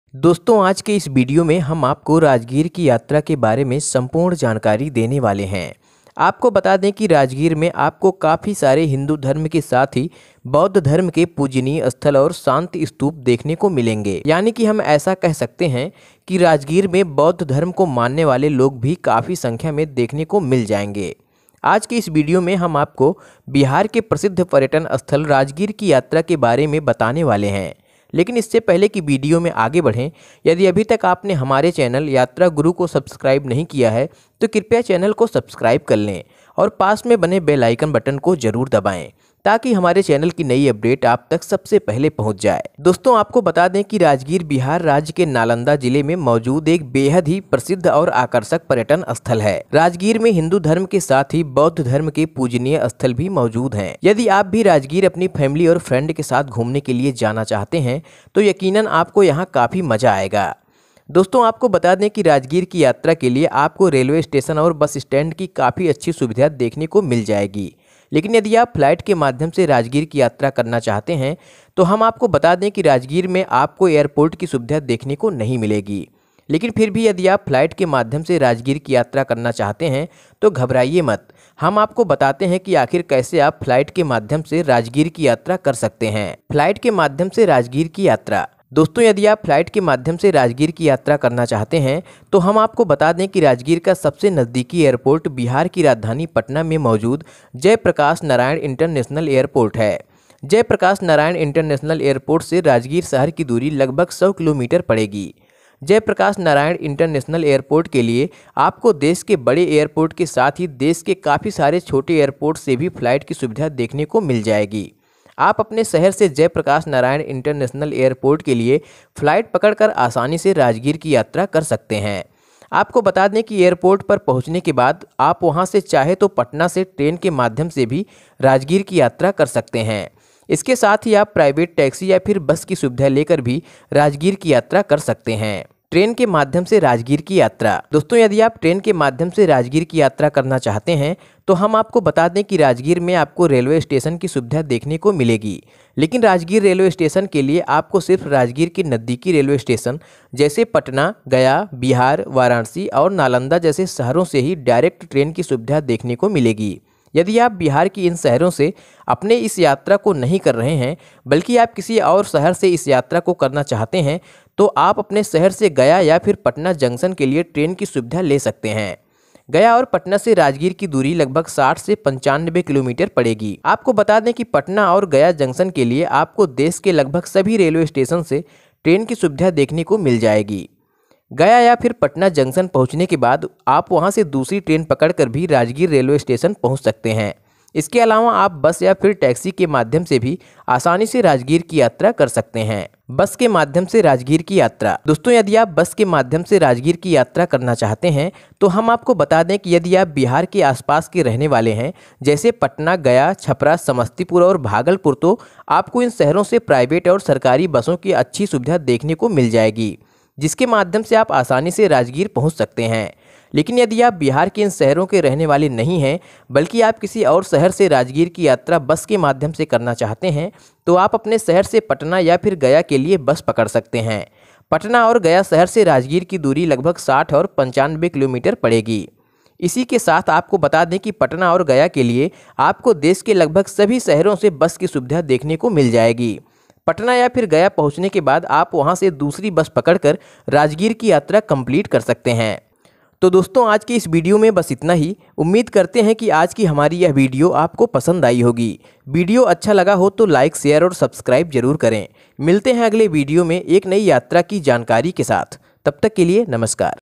राजगीर की यात्रा कैसे करें? || कहाँ घूमे,कहाँ ठहरे || rajgir tour guide
Rajgir Budget Tour Plan Guide | 2 Days @ RS 2000 | कहाँ घूमे,कहाँ ठहरे | Hotel,Food,Explore | Rajgir

Top 10 Tourist Places In Rajgir।Best Places To Visit In Rajgir Bihar।Bihar Tourism। Rajgir